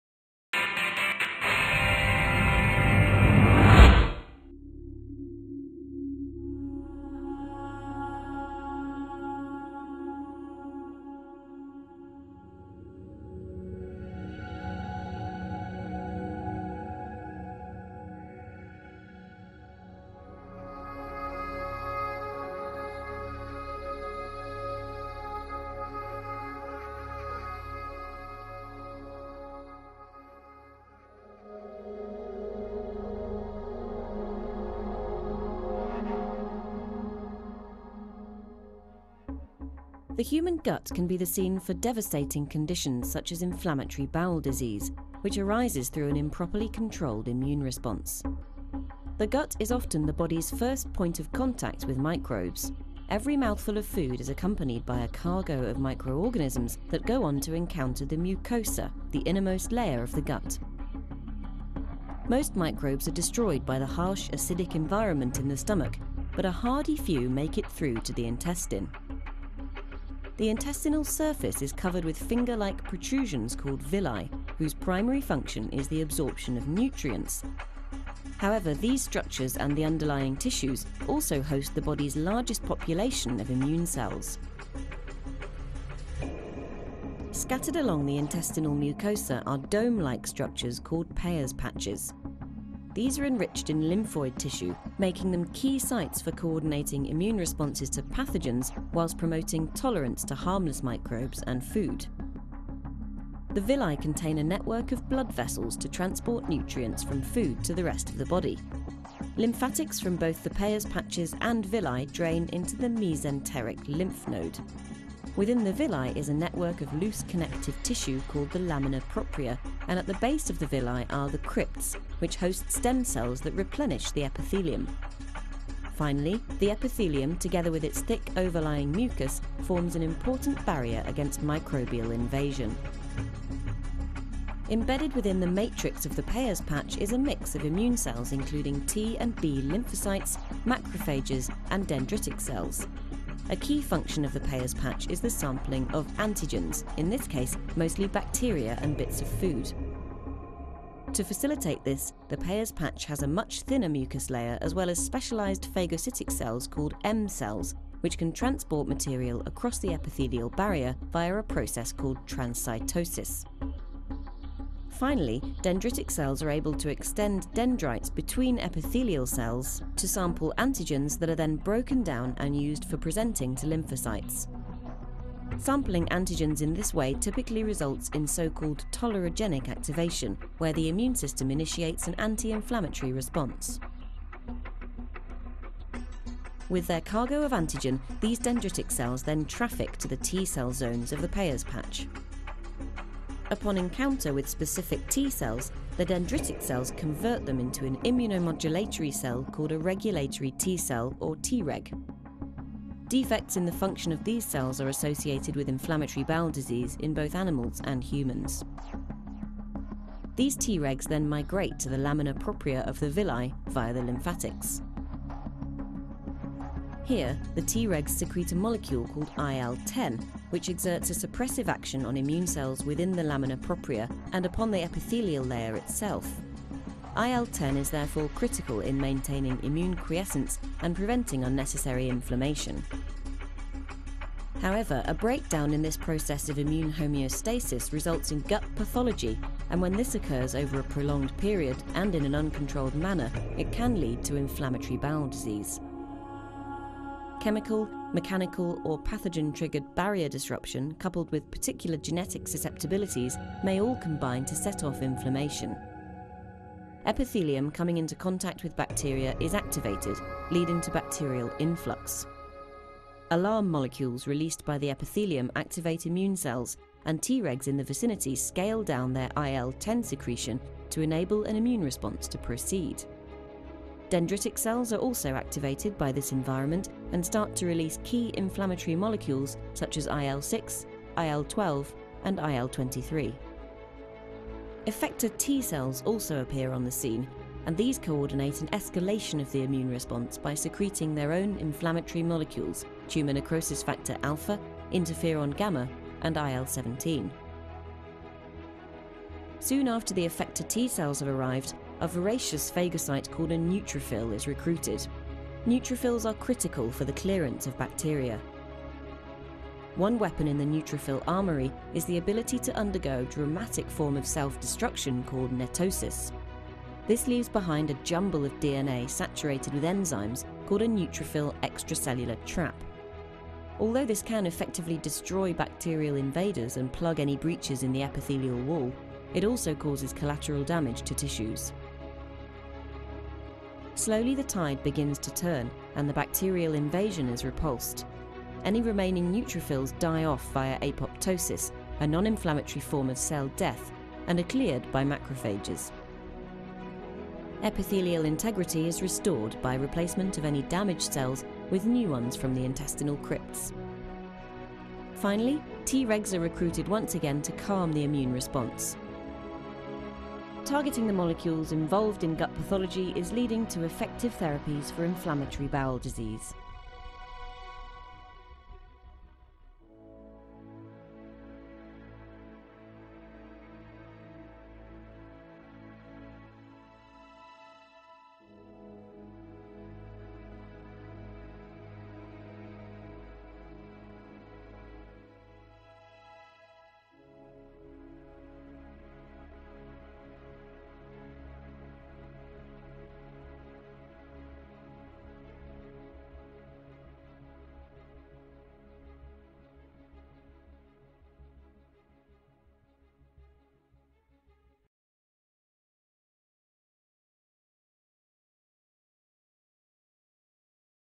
Immunology in the Gut Mucosa
The gut mucosa is the largest and most dynamic immunological environment of the body. It's often the first point of pathogen exposure and many microbes use it as a beachhead into the rest of the body. The gut immune system therefore needs to be ready to respond to pathogens but at the same time it is constantly exposed to innocuous environmental antigens, food particles and commensal microflora which need to be tolerated. Misdirected immune responses to harmless antigens are the underlying cause of food allergies and debilitating conditions such as inflammatory bowel disease. This animation introduces the key cells and molecular players involved in gut immunohomeostasis and disease.